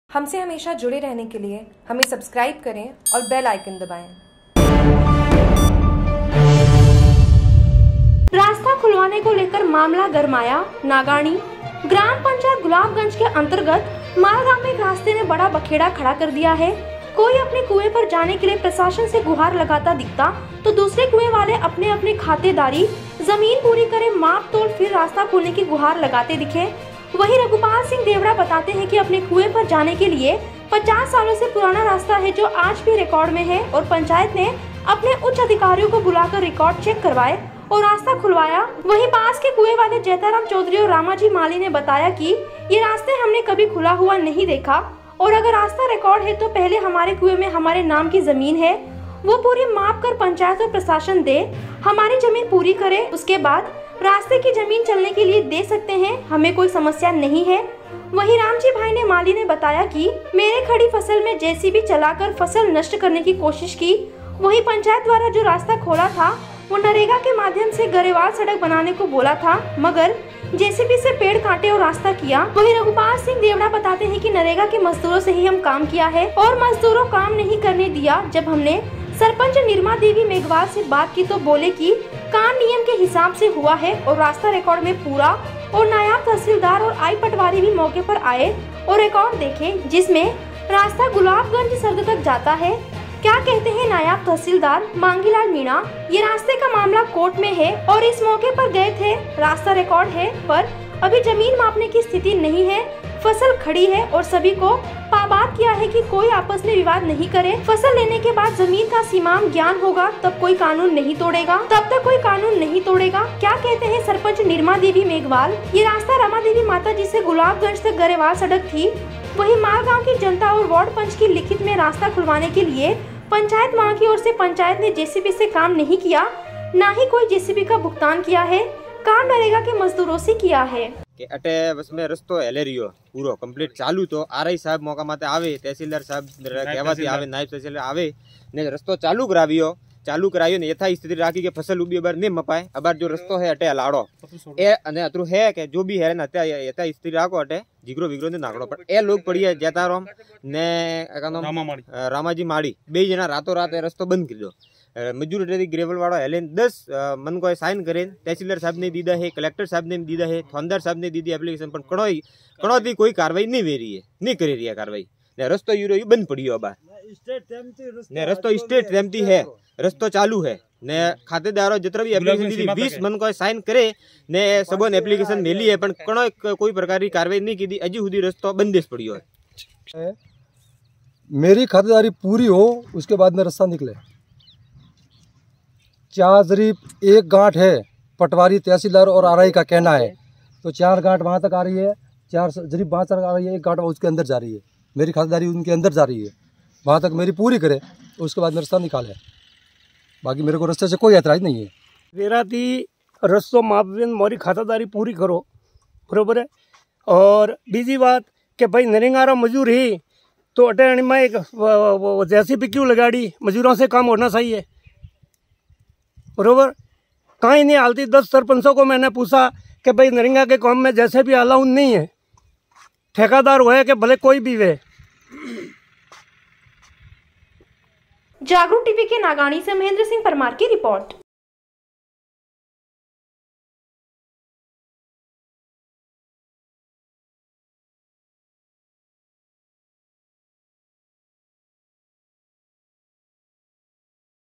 आबूरोड : रास्ता खुलवाने को लेकर मामला गरमाया
कोरोना की तीसरी लहर के चलते पहला वीकेंड कर्फ्यू रविवार को पूरी तरह से सफल रहा। बाजार में प्रतिष्ठानों के ताले नहीं खुले। वही आवाजाही
नगण्य रही। पुलिस प्रशासन मुस्तैद रहा। बेवजह बाजार में घूमते लोगों को रोककर पूछताछ की गई।
विओ: लगातार बढ़ते कोरोना संक्रमण के चलते राज्य सरकार की ओर से विकेट
कर्फ्यू के आदेश दिए गए। जिसके चलते रविवार को पहला वीकेंड कर्फ्यू पूरी तरह सफल रहा। बाजार में दुकानों के ताले नहीं खुले। आवश्यक वस्तुओं की दुकानें, मेडिकल स्टोर आदि खुले रहे। शहर के अंबाजी चौराहा, लुनियापुरा
ब्रिज, मानपुर चौराहा, स्टेशन चौराहा,पारसीचाल, पंचायत समिति के पास पुराना चेक पोस्ट चौराहा पर पुलिस मुस्तैदी से तैनात रही। बेवजह घूम रहे लोगों को रोका गया। बाहर निकले की वजह वाजिब होने पर ही आगे जाने की इजाजत दी गई। बिना मास्क लगाए वाहन चालक पुलिस को देखते ही यू-टर्न लेते दिखाई दिए।